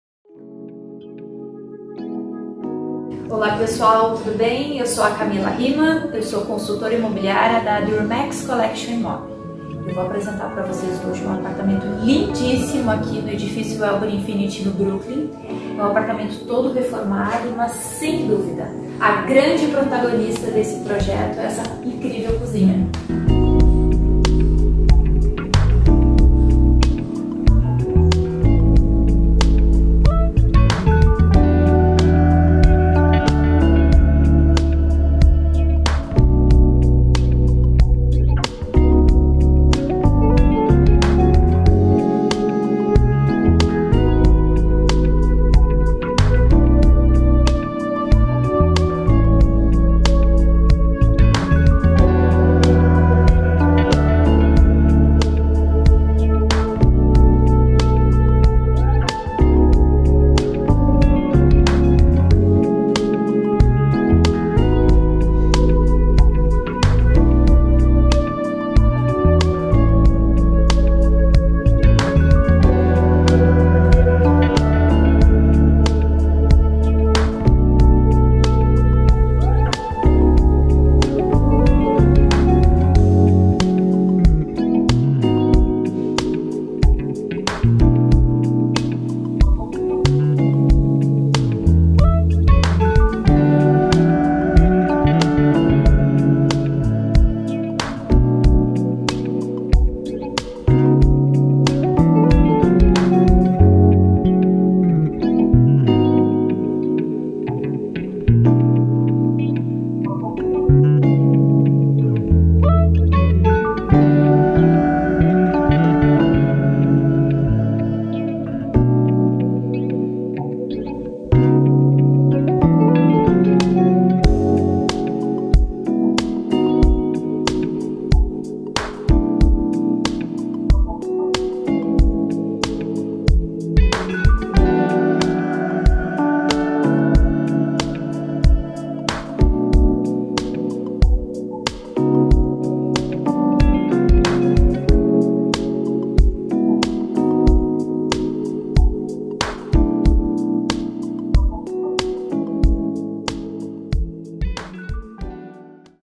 Apartamento 94m² Brooklin - São Paulo/ SP
Sabe aquele imóvel que você se apaixona a primeira vista?
É exatamente assim o apartamento que iremos apresentar...

Descolado, colorido e de muito bom gosto em suas escolhas, assim eu o descreveria.
São 94m², 1 suíte, com um segundo dormitório reversível e 2 vagas de garagem.

A grande protagonista sem dúvidas é a cozinha. Em tons escuros, ela traz amplitude e integra mais ambientes como sala de estar e varanda gourmet. Com churrasqueira a carvão e vista para a Ponte Estaiada, essa unidade trará a você e família muito conforto e felicidades.

Prédio com lazer completo, sendo academia, piscina aquecida, quadra poliesportiva, sauna e brinquedoteca.

Agende sua visita e irá se surpreender...